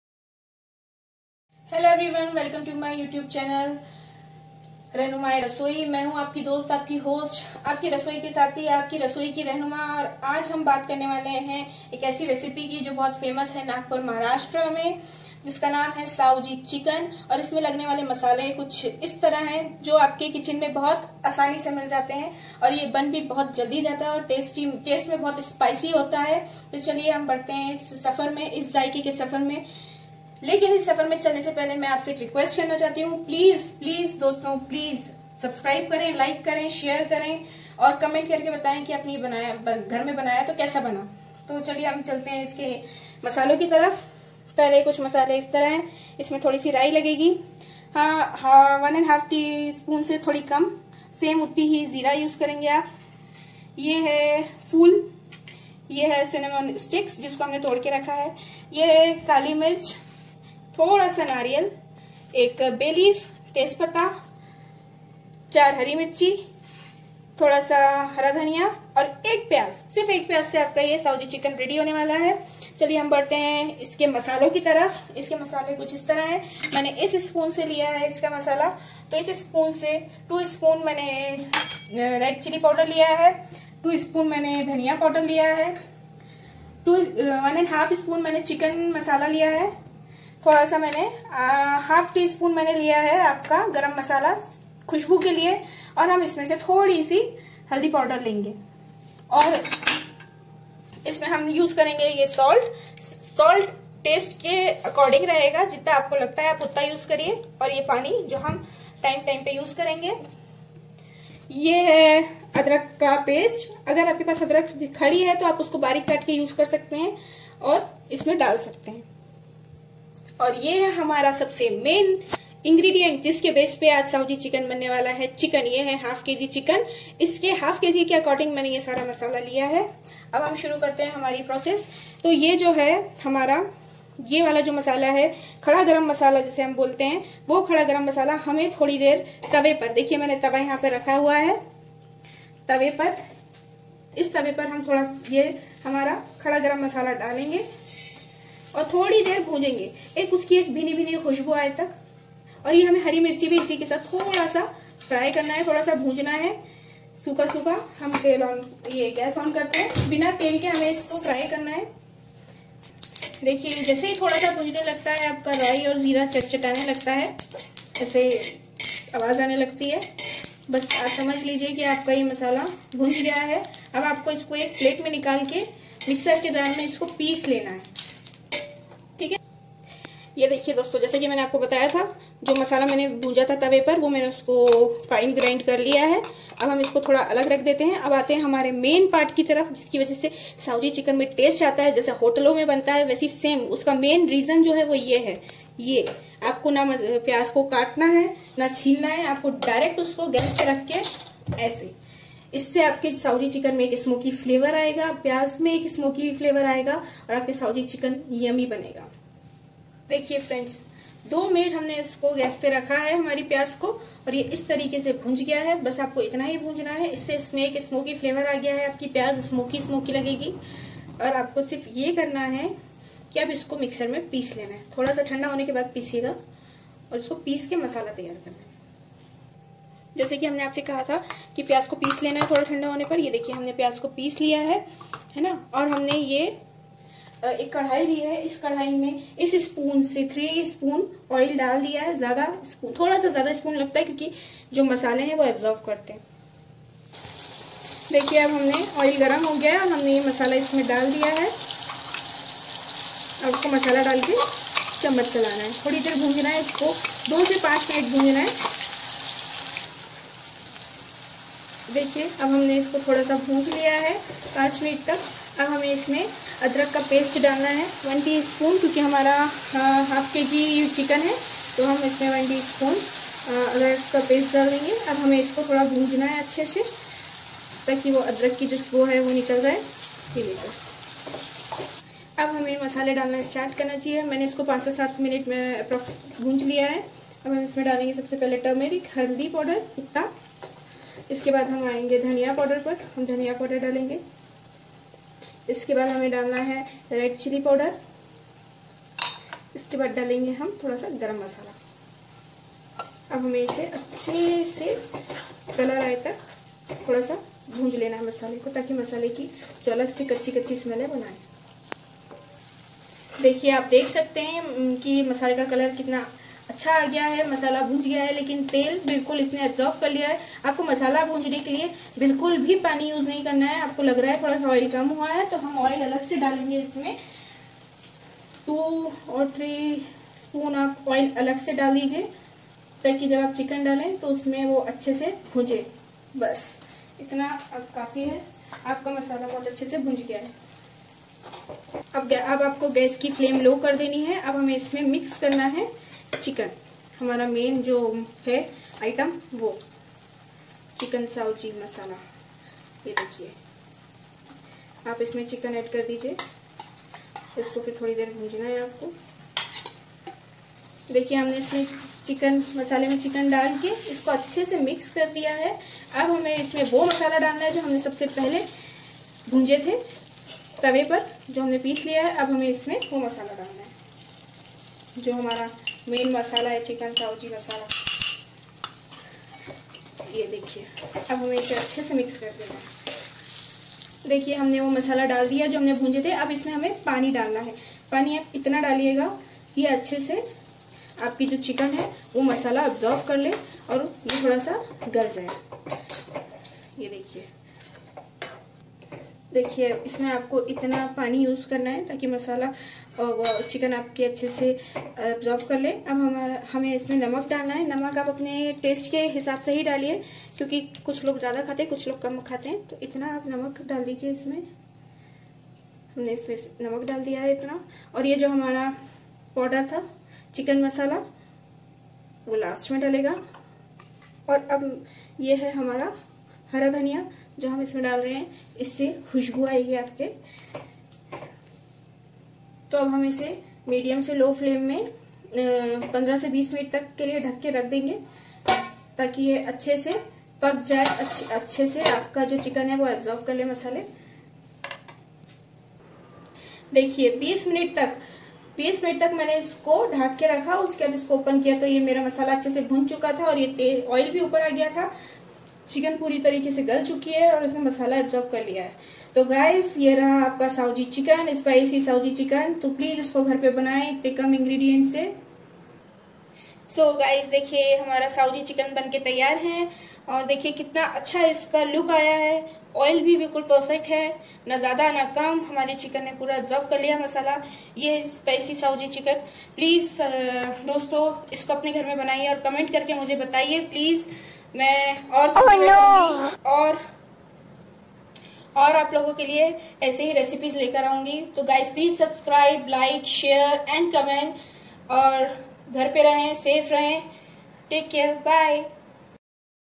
SAOJI CHICKEN | UMRED FAMOUS | SPECIAL SAOJI MASALA | उमरेड फेमस साओजी चिकन
Nagpur special Chicken saoji with spicy taste and mouth watering look !!!